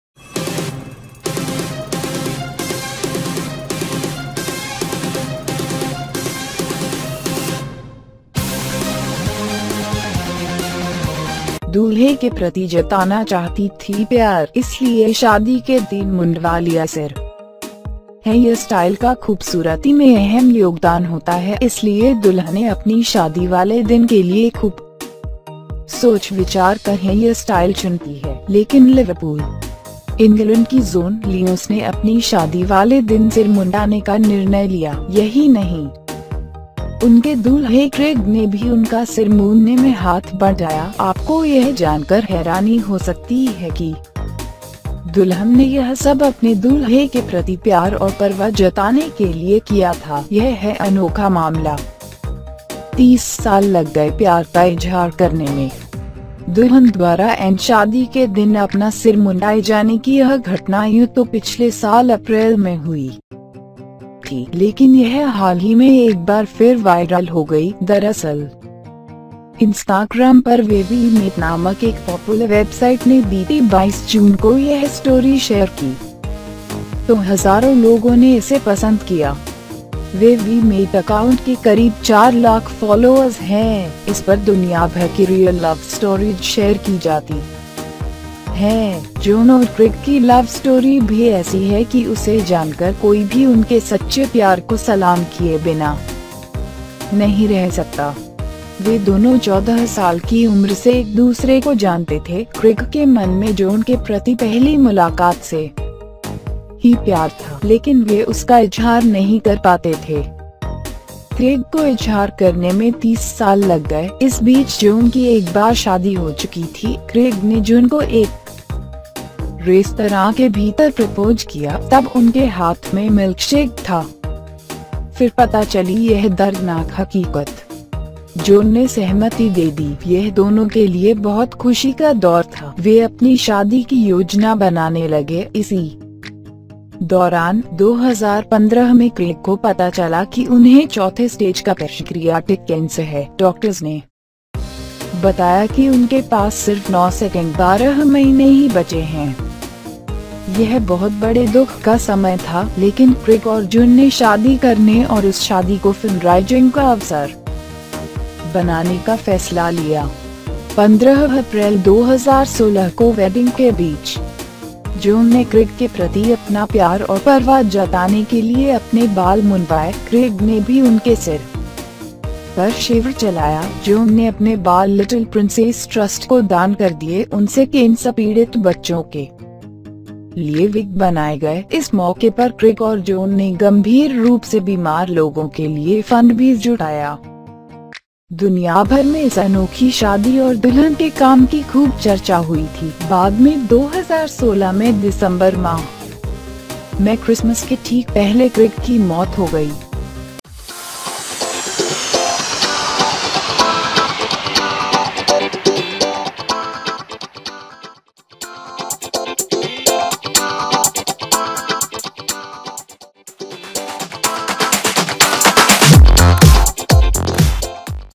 दूल्हे के प्रति जताना चाहती थी प्यार, इसलिए शादी के दिन मुंडवा लिया सिर || News For You
➤ दूल्हे के प्रति जताना चाहती थी प्यार, इसलिए शादी के दिन मुंडवा लिया सिर || 5 Countries In World Where Abortion Is Legal || News For You
हेयरस्टाइल का खूबसूरती में अहम योगदान होता है। इसलिए दुल्हनें अपनी शादी वाले दिन के लिए खूब सोच-विचार कर हेयरस्टाइल चुनती हैं। लेकिन लिवरपूल, इंग्लैंड की जोन लियोंस ने अपनी शादी वाले दिन सिर मुंडाने का निर्णय लिया। यही नहीं, उनके दूल्हे क्रेग ने भी उनका सिर मूंडने में हाथ बंटाया। आपको यह जानकर हैरानी हो सकती है कि दुल्हन ने यह सब अपने दूल्हे के प्रति प्यार और परवाह जताने के लिए किया था।
➤ My Channel: News For You
➤ Video News Source: From Internet News Site